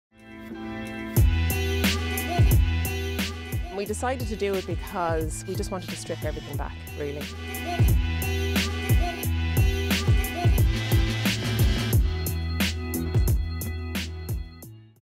Innovation and experimentation are key to our success
Distillery Manager Lisa Ryan talks about our exciting new project. All will be revealed. Watch this space.